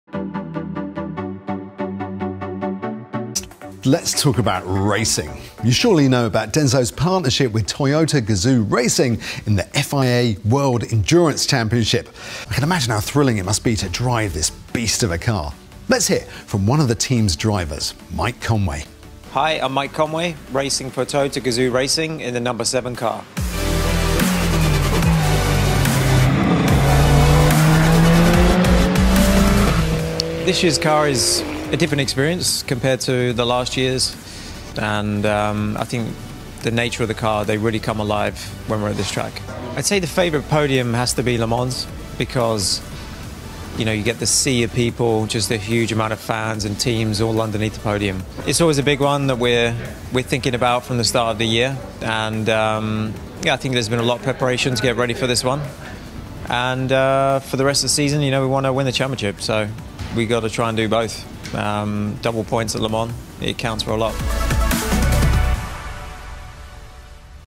𝗗𝗘𝗡𝗦𝗢 𝗺𝗮𝗶𝗻 𝘀𝗽𝗼𝗻𝘀𝗼𝗿 𝗼𝗳 𝘄𝗶𝗻𝗻𝗶𝗻𝗴 𝘁𝗲𝗮𝗺 𝗶𝗻 𝗟𝗲 𝗠𝗮𝗻𝘀 𝟮𝟰 𝗵𝗼𝘂𝗿𝘀 𝗿𝗮𝗰𝗲
Did you know, that DENSO, which means “electrical element” in Japanese, was once part of the Toyota Group and manufactured automotive components?